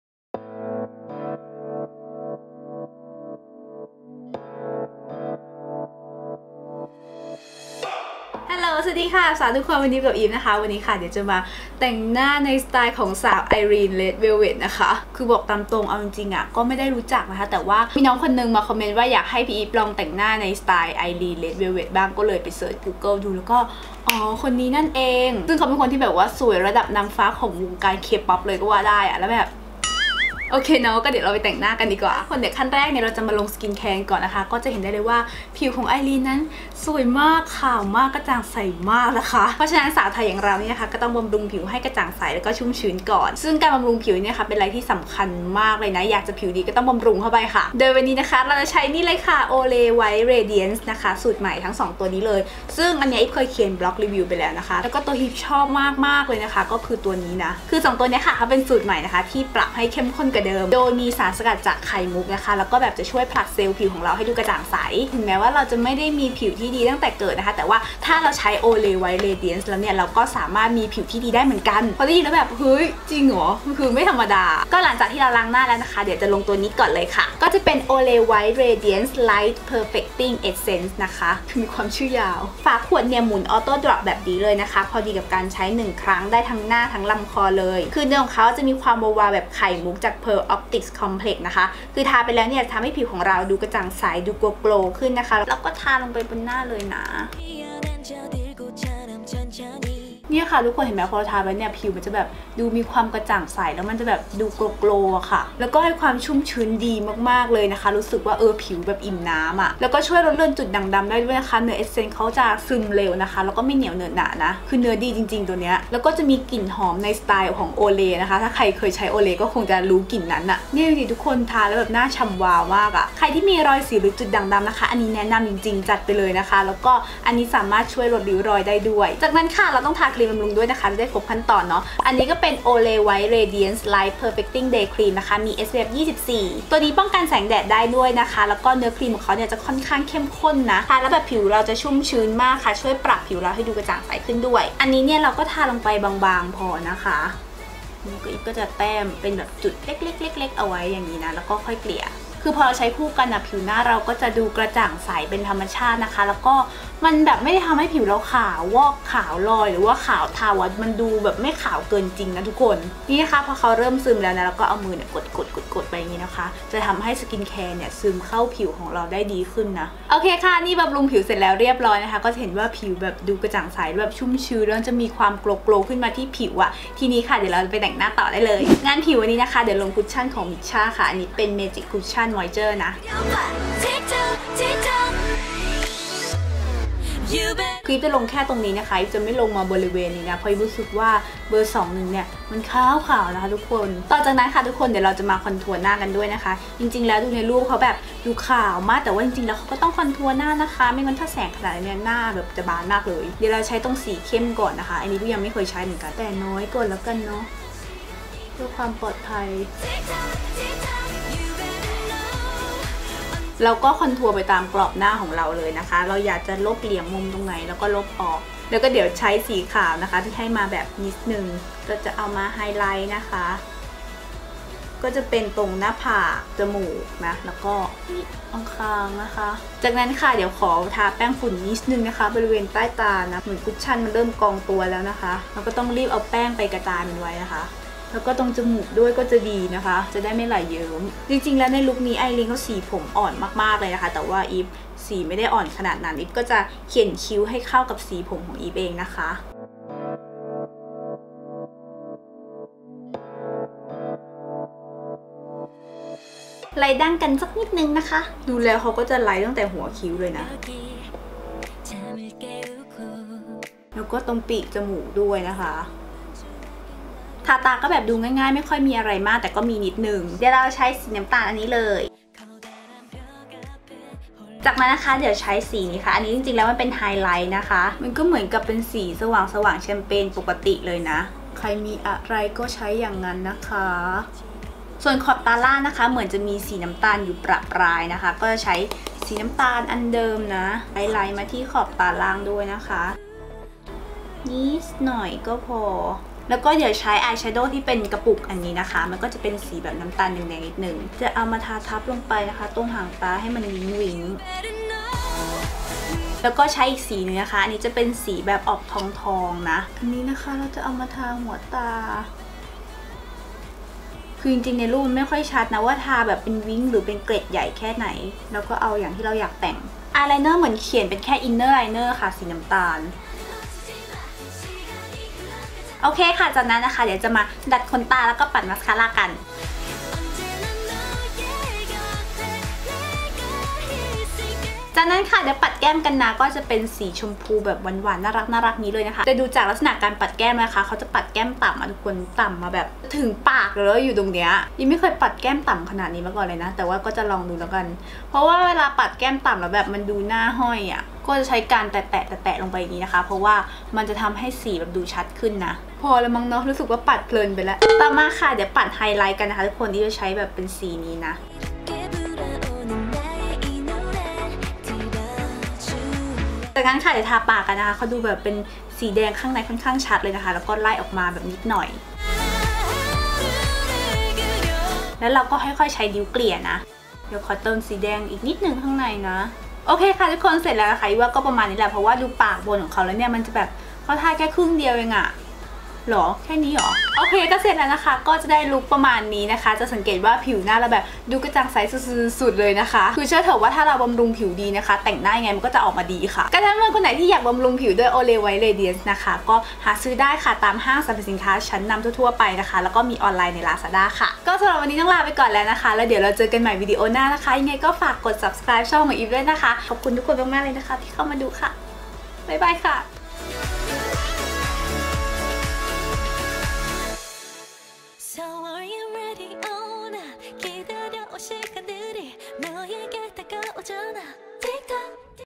ทำไงให้หน้ากระจ่างใสแบบสาวเกาหลี+แต่งหน้าสไตล์Irene Red Velvet║Evefee
มาๆแต่งหน้าสไตล์ Irene Red velvet กันหน่อยดีกว่า
แต่ก่อนที่จะแต่งหน้านั้นน เราก้จะต้องบำรุงผิวกันสักนิดด้วย Olay White Radiance นะคะ ผิวจะได้กระจ่างใสก่อนแต่งหน้า

Olay White Radiance สูตรปรับปรุงใหม่ สามารถหาซื้อได้ที่ Watsons, boots, Eveandboy และ ซุปเปอร์มาร์เก็ตชั้นนำทุกแห่ง หรือทางออนไลน์ Lazada
ดูข้อมูลสินค้าเพิ่มเติมที่